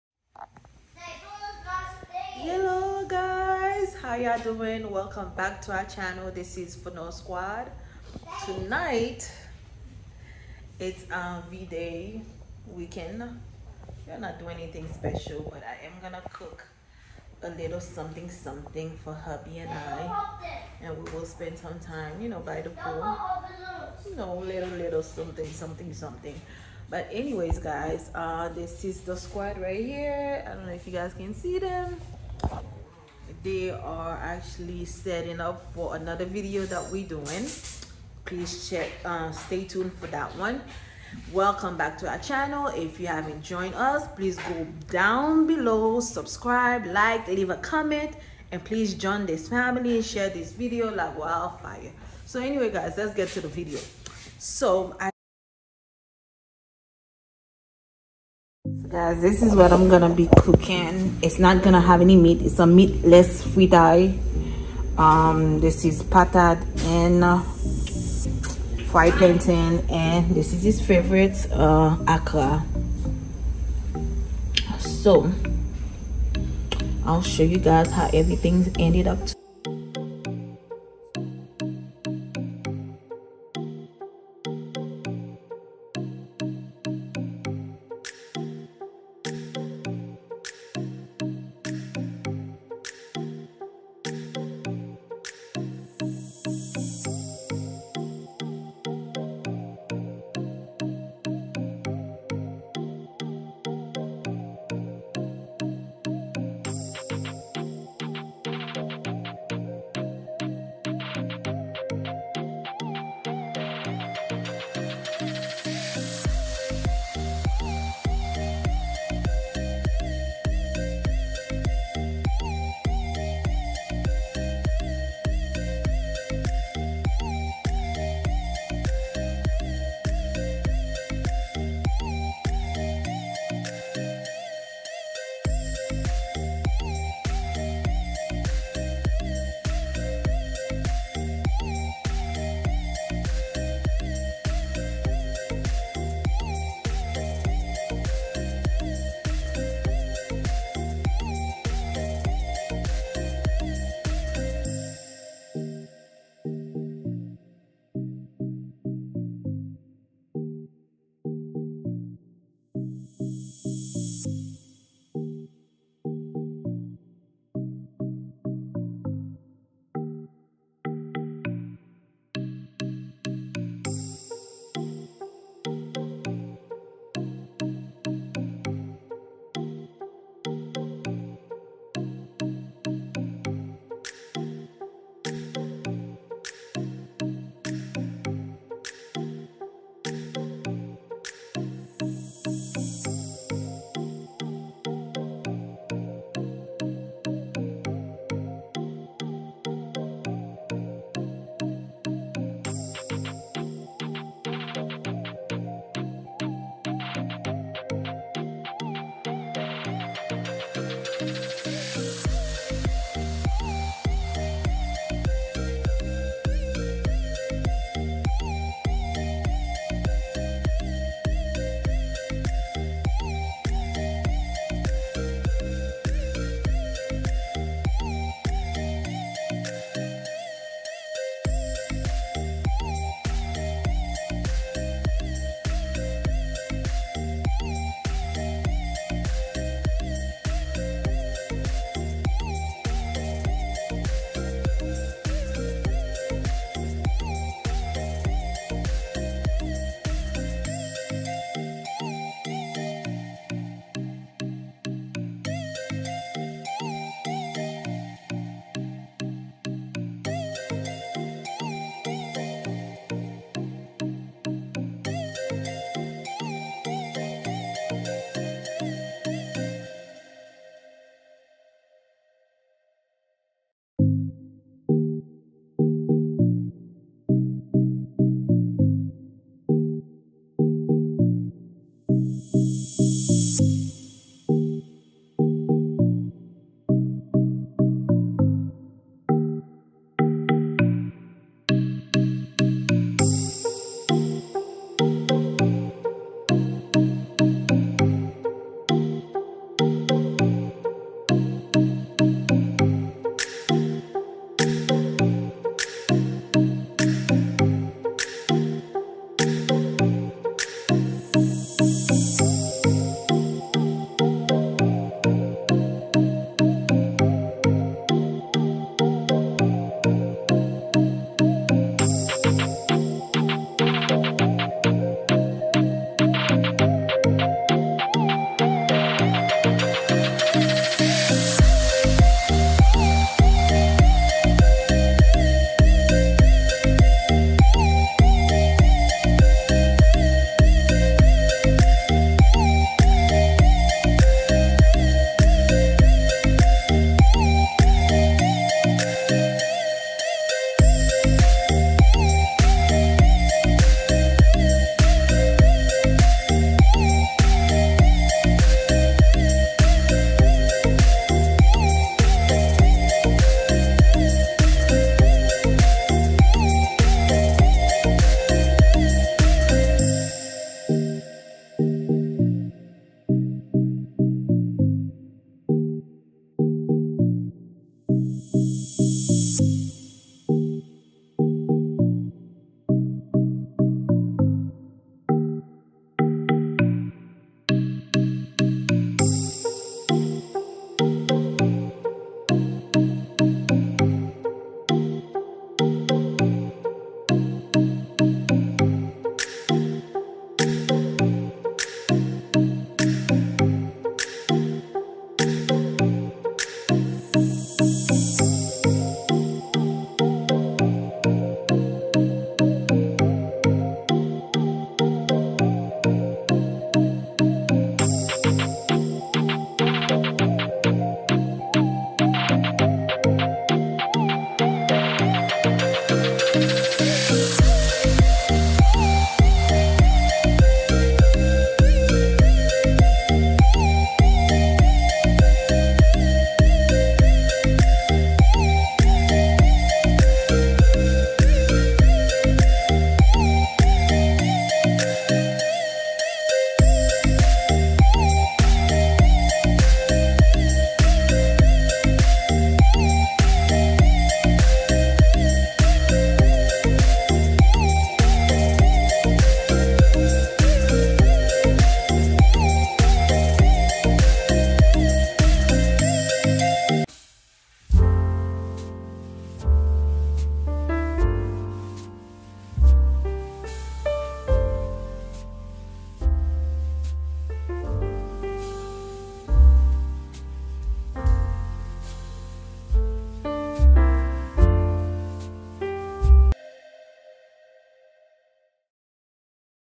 OUR LITTLE ROMANTIC DINNER.(VDAY/with our kids)!!!
check out our little romantic  dinner the kids put together for us